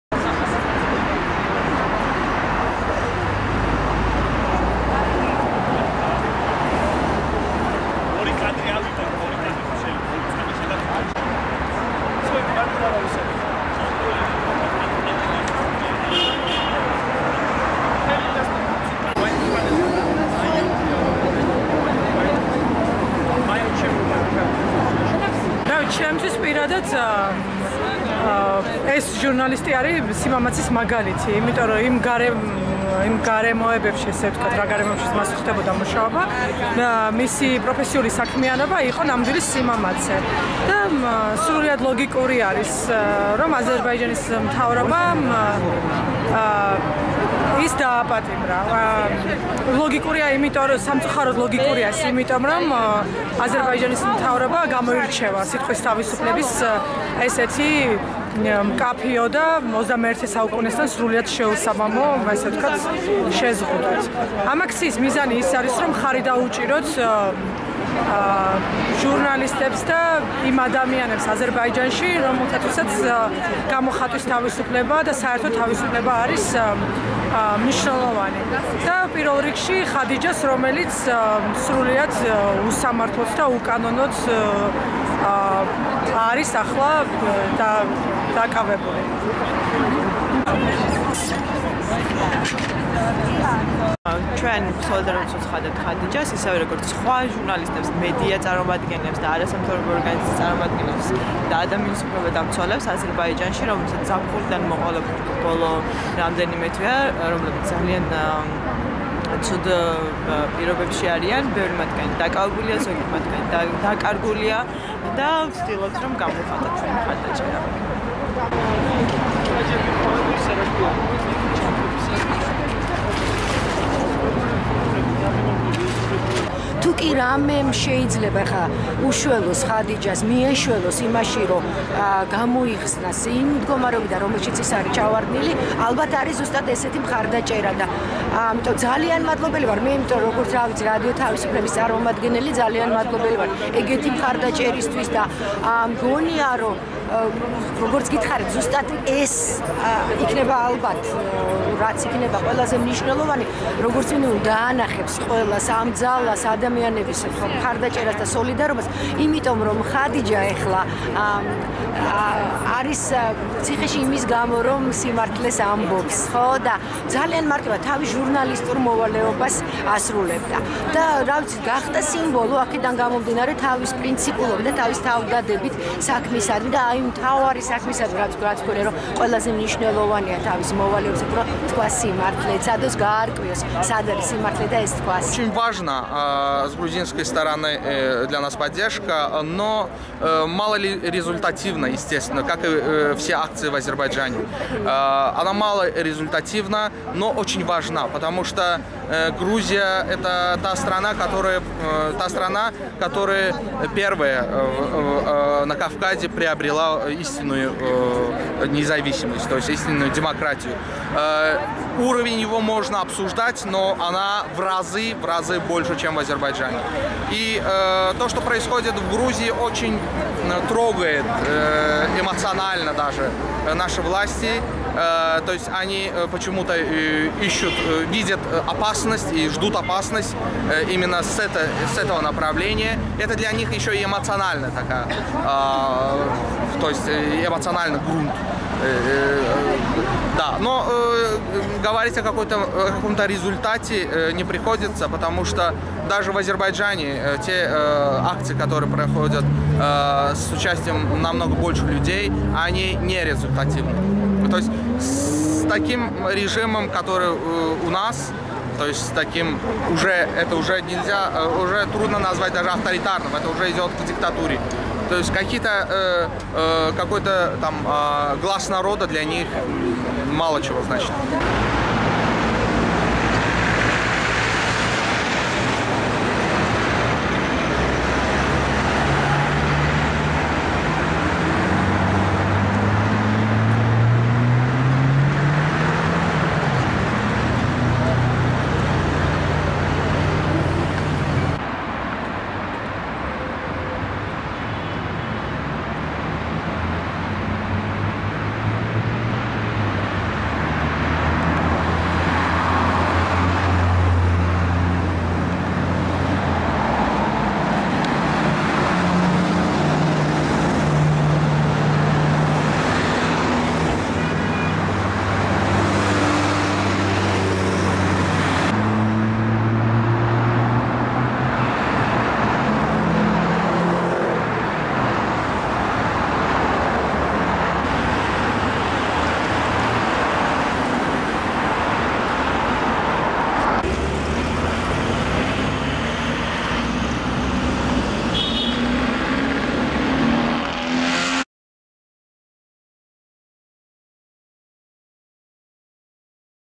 ხადიჯა ისმაილოვას მხარდასაჭერი აქცია თბილისში
2014 წლის 10 დეკემბერს თბილისში, აზერბაიჯანის რესპუბლიკის საელჩოსთან, საქართველოში მოქმედი მედიასაშუალებებისა და არასამთავრობო ორგანიზაციების წარმომადგენლების ორგანიზებით, სოლიდარობის აქცია გაიმართა  ბაქოში დაპატიმრებული გამომძიებელი ჟურნალისტის - ხადიჯა ისმაილოვას მხარდასაჭერად.

Protest of representatives of Georgian Media and NGOs at the Embassy of Azerbaijan in Tbilisi, to support investigative journalists Khadija Ismaylova, recently arrested in Baku. 10/12/2014

Акция протеста представителей СМИ и НПО у здания посольствы Азербайджана в Тбилиси для поддержки журналиста Жадиджа Исмайлова, которая была арестованна в Баку. 10/12/2014